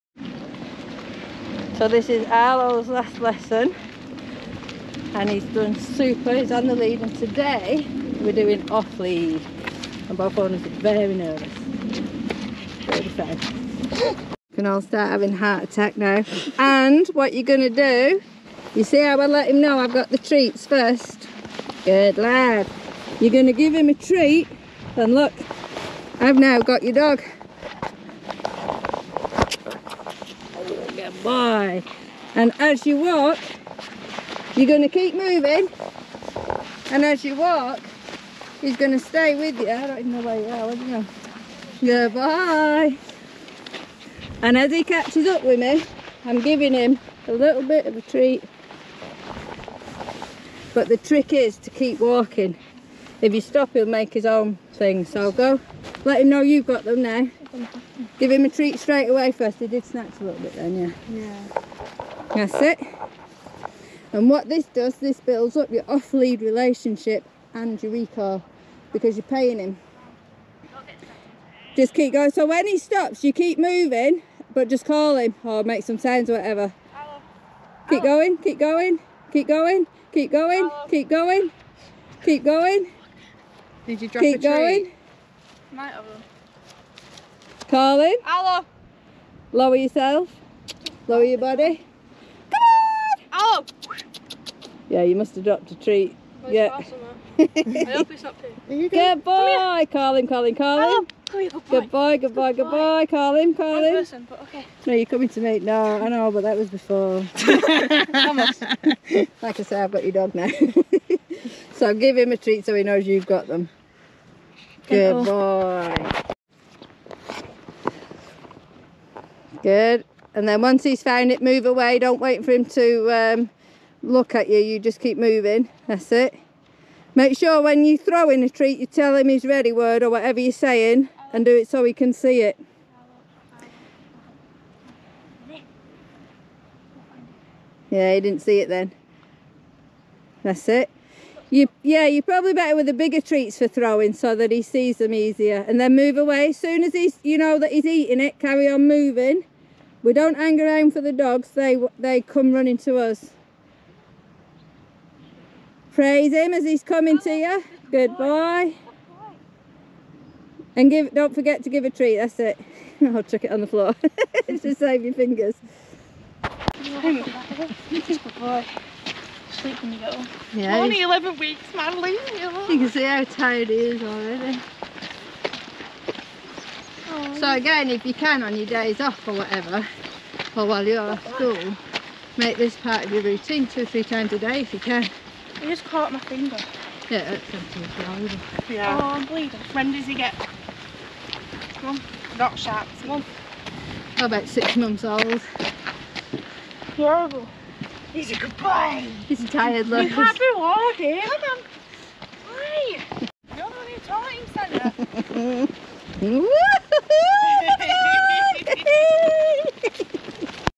11 wk gsd puppy Arlo, 1st time off lead!!
both owners were nervous at letting him off lead but they did super!! (just avoid the sheep poop!) and little Arlo was zonked out when he got home! ❤️